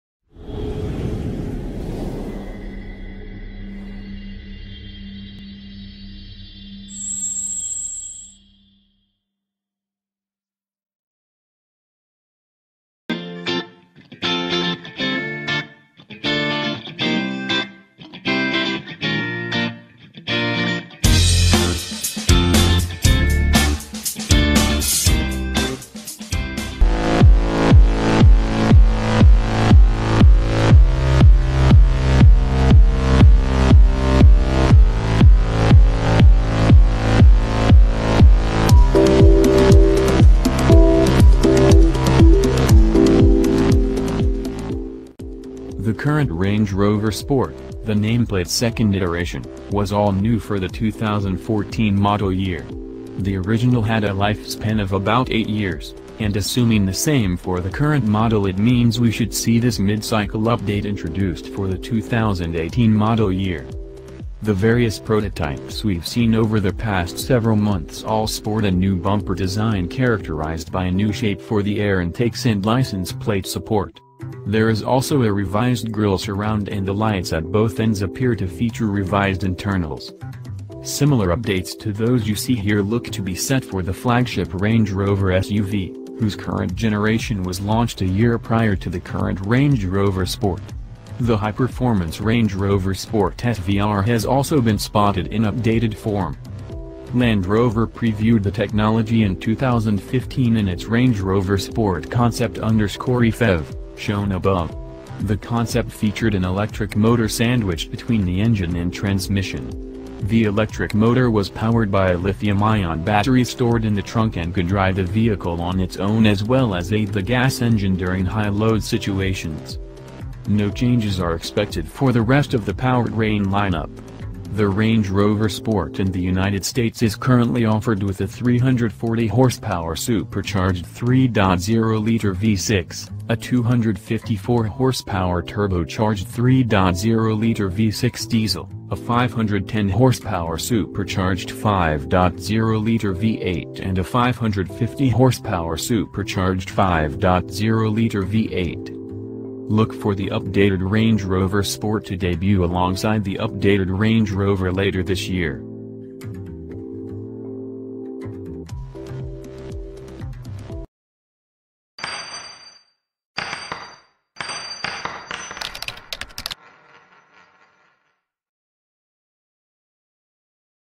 THE BEST !! 2018 RANGE ROVER SPORT SVR REVIEW
2018 RANGE ROVER SPORT SVR REVIEW - Land Rover is working on a mid-cycle update for its Range Rover Sport, and naturally the updates will filter across to the high-performance SVR variant.
We have new spy shots of a prototype for the updated Range Rover Sport SVR, a vehicle we expect to be launched alongside the updated version of the regular Range Rover Sport sometime next year. They should arrive as 2018 models.